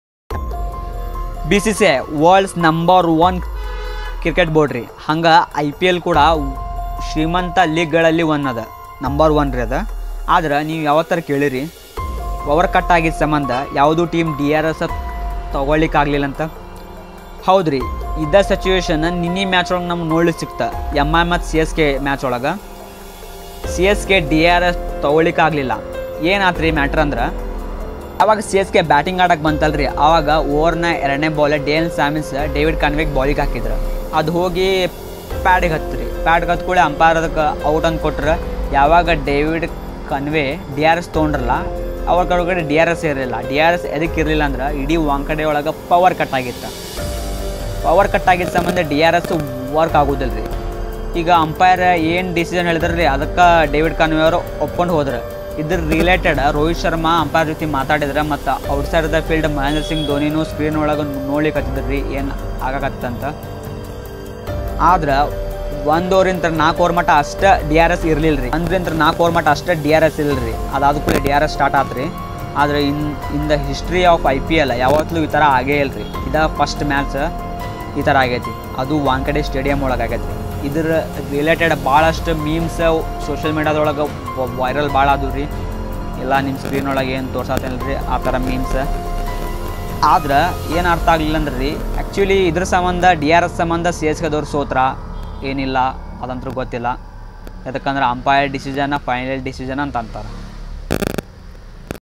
TATA IPL Match MI VS CSK | WHAT HAPPENED TO DRS | IPL ನಲ್ಲಿ DRS ಇಲ್ಲ 🤦| Power Cut During match | MCG
IPL Match MI VS CSK | WHAT HAPPENED TO DRS | IPL ನಲ್ಲಿ DRS ಇಲ್ಲ 🤦| Power Cut During Match in Wankhede  Stadium in Mumbai |

Welcome To Channel  🖐️🖐️MCG

In The Match no 59 MI Vs CSK . CSK and MI did not had DRS for the first two overs of the first innings due to a Power Cut at the  Wankhede Stadium on Thursday. The Toss Was also Delated by Electricity issue at the Stadium.

Note :- This video is only for educational and entertainment purpose ! _____________________________________ Photos & Videos from shutterstock.com , videvo and Google